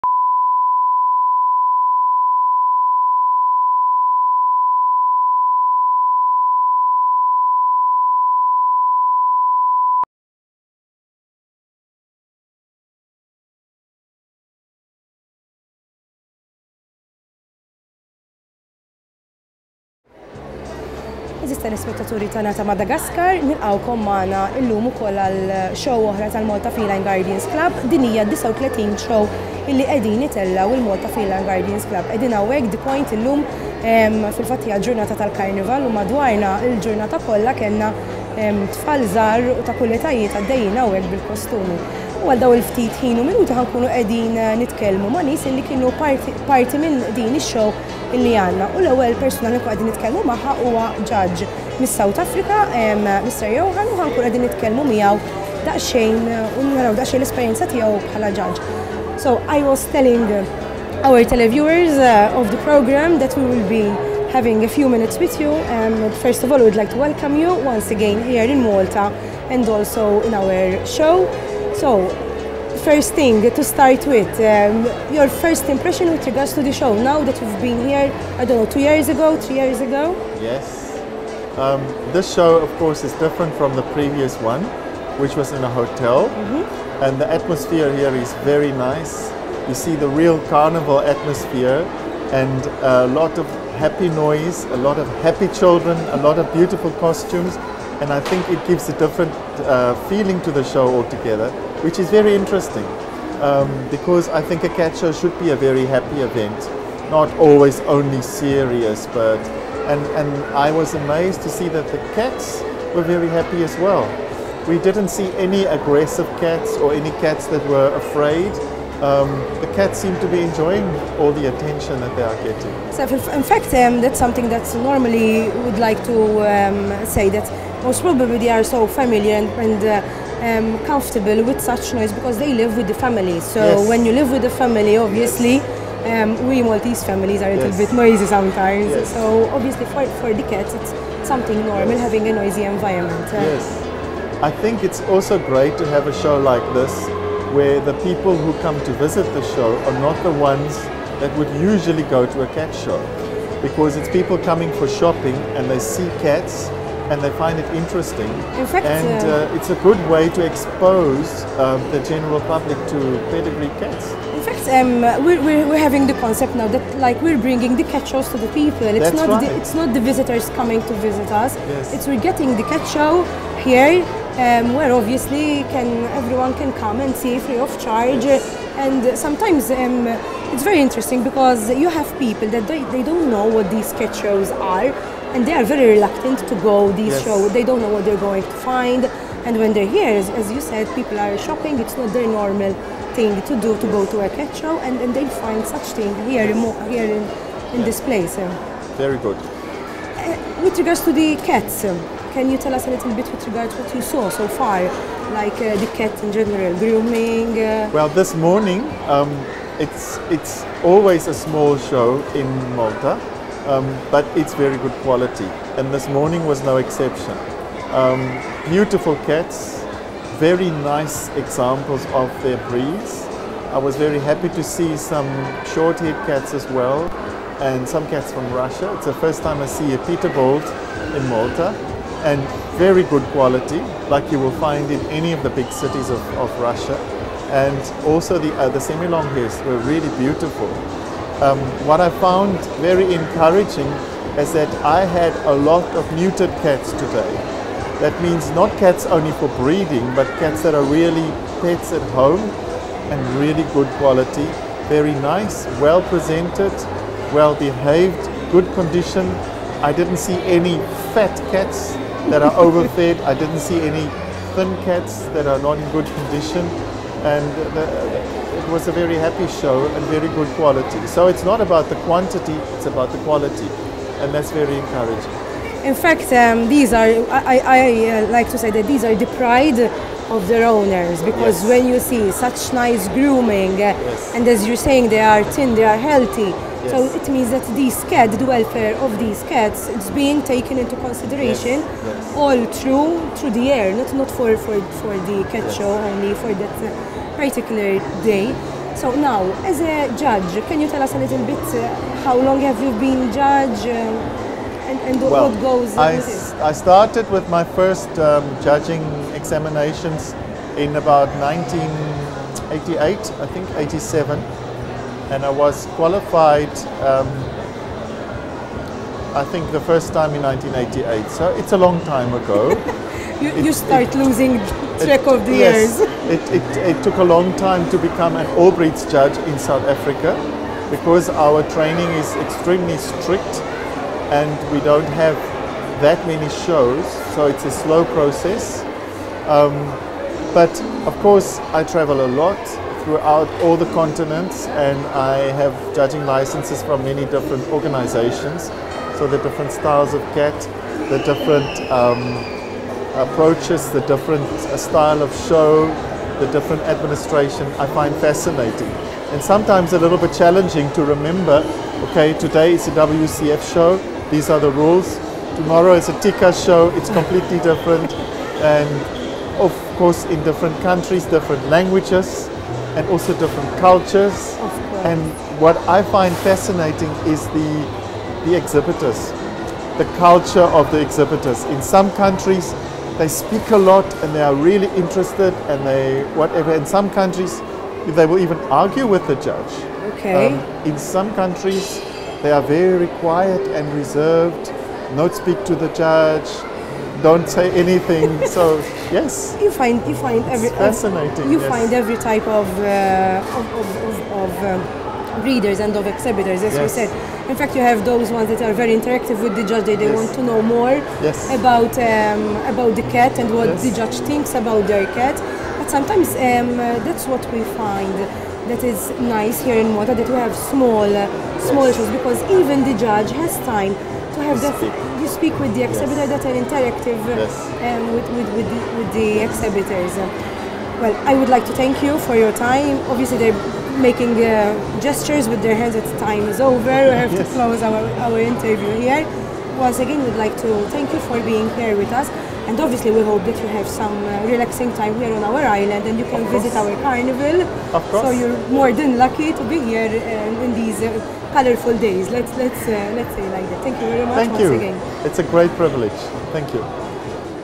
Mr. Johann Lamprecht, South Africa (AB,WCF) - MFGC 39th International Cat Show - February 2013
What a day ! Another International cat show is over and we as a club are very satisfied with the numerous amount of people that came and visit our show at Tigne Point last Saturday. Obviously as the show is over we have the winners that won the two General Best and the Super General Best respectively. The first show winner was a magnificent persian, Alasca dell' Arcobaleno owned by Eleonora Varcellino from Italy. The second show winner was a Maine coon cat - Sryamainecoon Oty owned by Roberta Micallef. These 2 cats competed together for the super general best and the white Persian cat was awarded the Super General Best. Other awards were given out to the Best neutered cat, Best kitten, Best adult and Best domestic cat won by PR. Diciotto Carati Nuance, Micabliss Savana, CH. Micabliss Soleil and Mittens. The Best cat of 2012 was awarded to CH. Master IT* Radiosa Del Falco D'oro.
The MFGC comittee would like to thank the three judges present on the day. These were Johann Lamprecht and Thea Lamprecht from South Africa. The third judge was Marco Pozzi from Italy. They did a marvellous job while judging these cats. We would also like to thank all the visitors that visited the show. Another big thanks goes also to all the helpers and the sponsors that without these helping hands a show like this cannot be possible.